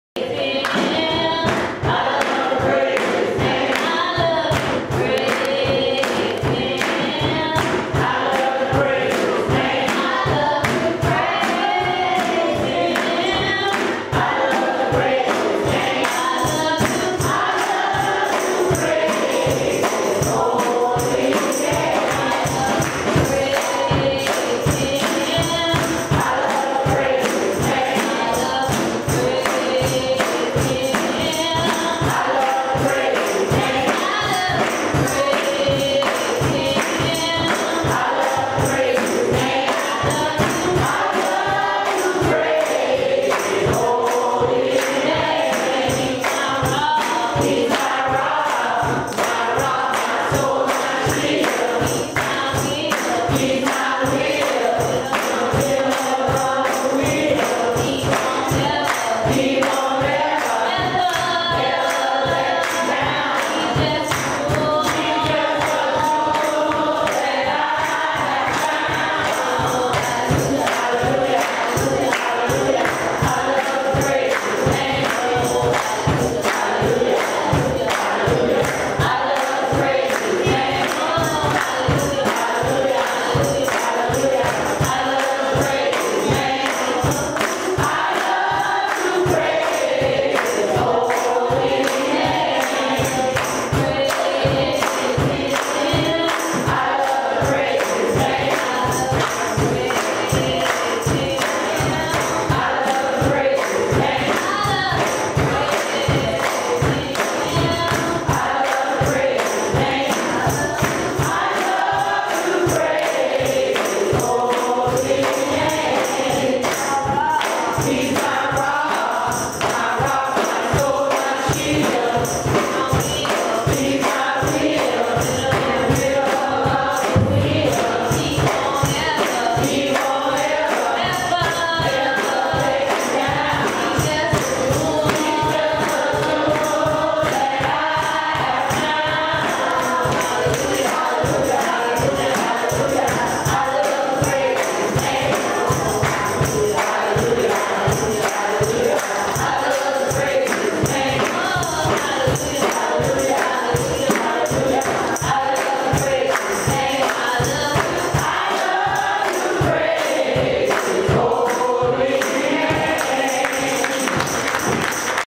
Mount Mission MB Church Featuring Deacon Harvey Jones 10-24-10 v2
Mount Mission Missionary Baptist Church -- 810 George W. Engram Blvd. -- Daytona Beach, Florida - 32114 - Church Phone: - Dr. Robert Lewis, Sr., Pastor Featuring Deacon Harvey Jones -- October 24, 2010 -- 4th Sunday Morning Services.
Vi 2